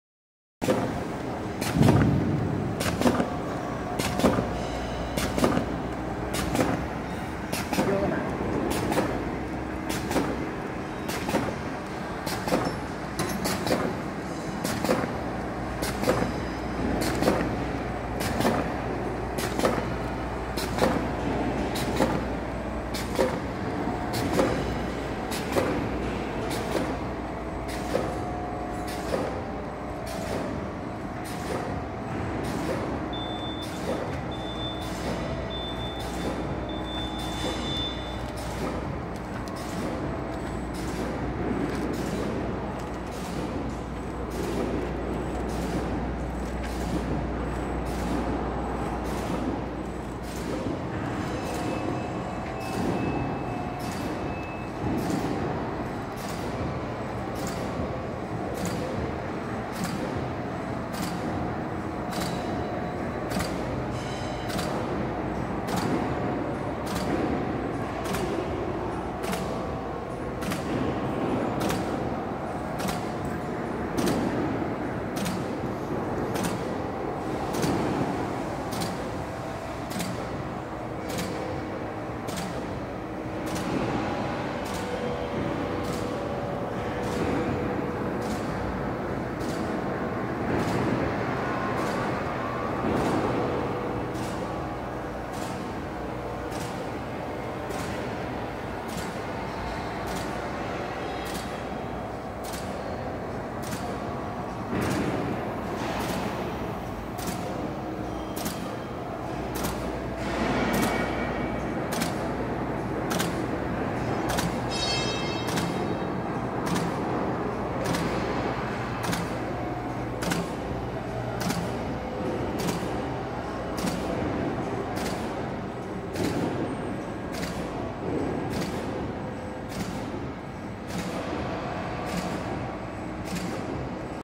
CWZD-C+FD 2 Lines doypack pouch machine with cutting device
This machine is customized model for making 2 lines doypack pouches either by third film inserted or by single web folded. and with optional die cutting device for shaped bag.